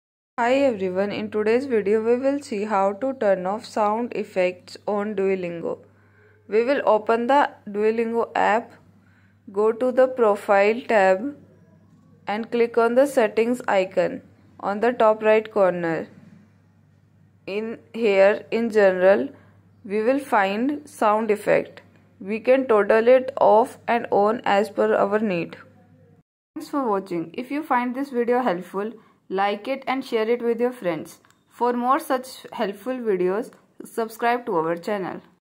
How To Turn Off Sound Effects On Duolingo
In this tutorial video, you will learn how to disable sound effects on Duolingo. Follow the step-by-step instructions to easily turn off the sound effects and customize your learning experience on the language learning platform.